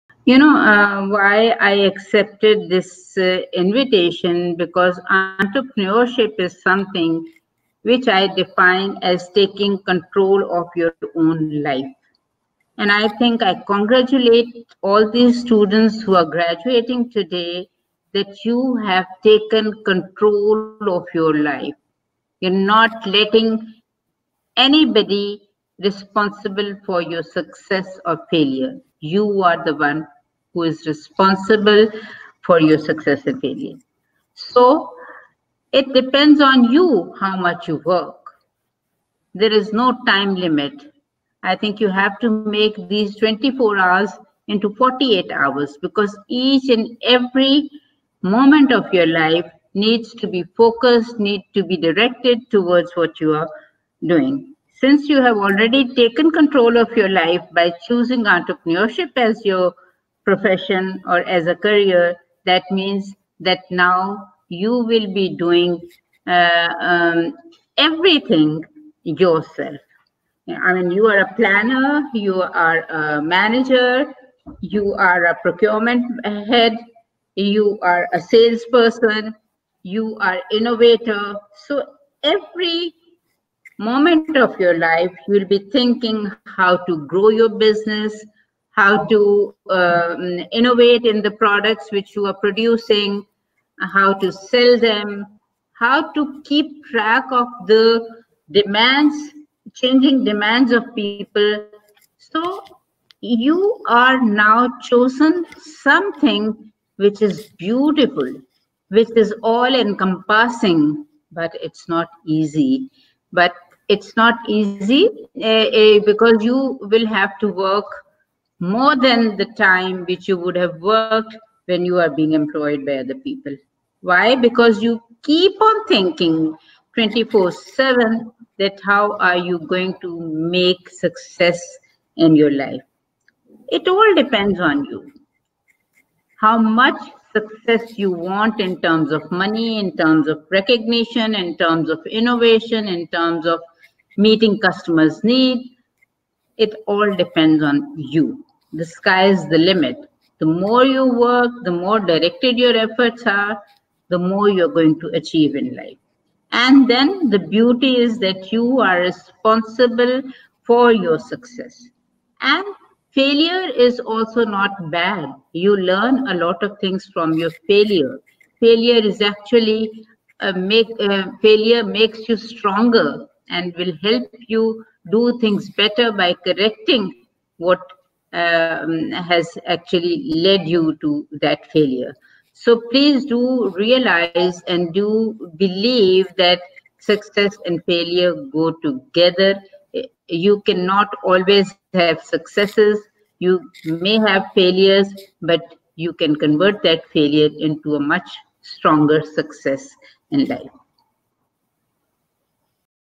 Tahira Reza President First Women Bank - Startup Pakistan Training Graduation Event July 11, 2020
Experienced entrepreneurs, please join as a mentor to support Startup Pakistan